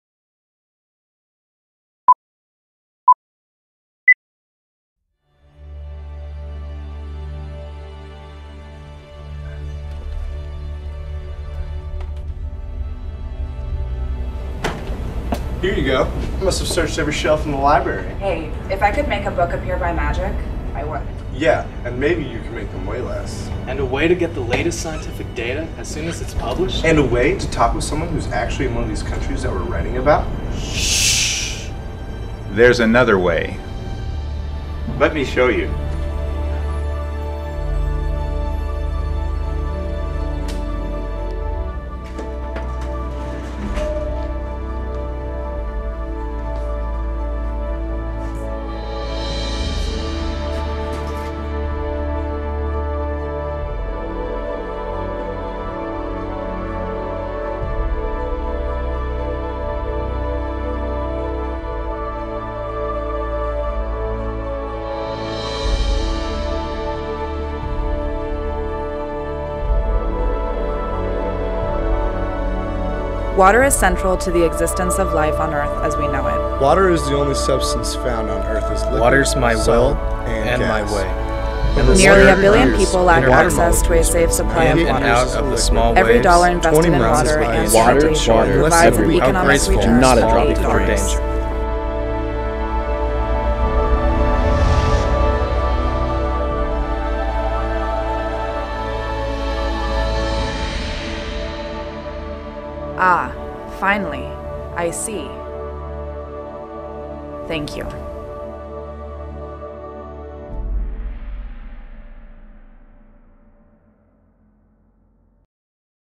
New SU Library
Seattle University students prepare to see the new world of research and learning to be made possible by a $55 million library renovation.